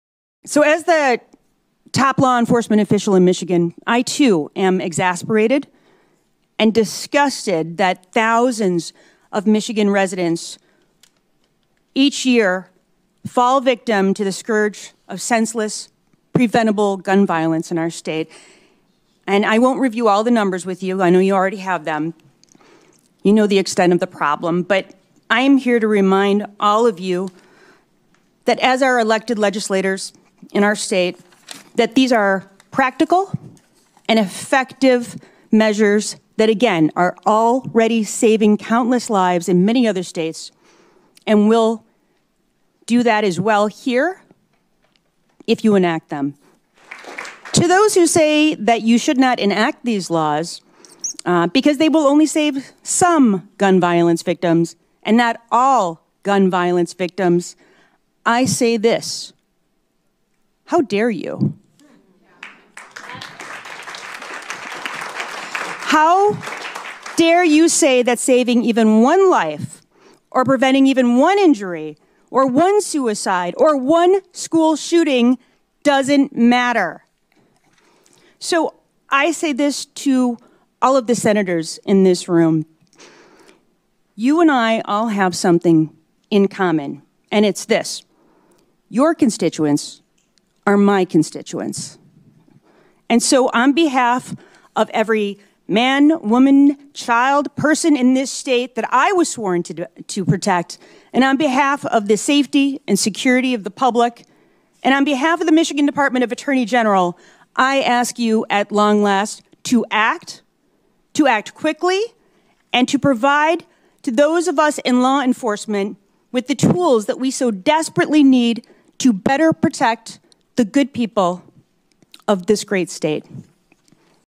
Attorney General Dana Nessel Testifies in Support of Gun Safety Legislation
Attorney General Dana Nessel testified before the Senate Civil Rights, Judiciary, and Public Safety Committee on March 2, 2023.

“As the top law enforcement agent in the state of Michigan, and as a mother, I too am exasperated and disgusted that thousands of Michigan residents fall victim each year to the scourge of senseless, preventable gun violence in our state,” Nessel testified. “These are practical and effective measures that are already saving lives in other states and will here as well. In the event that they are challenged in court, it would be my duty and honor to defend them. I pledge that when these laws are enacted, I will use every tool of my office to ensure that every Michigan resident is informed about these laws and that they will be vigorously enforced.”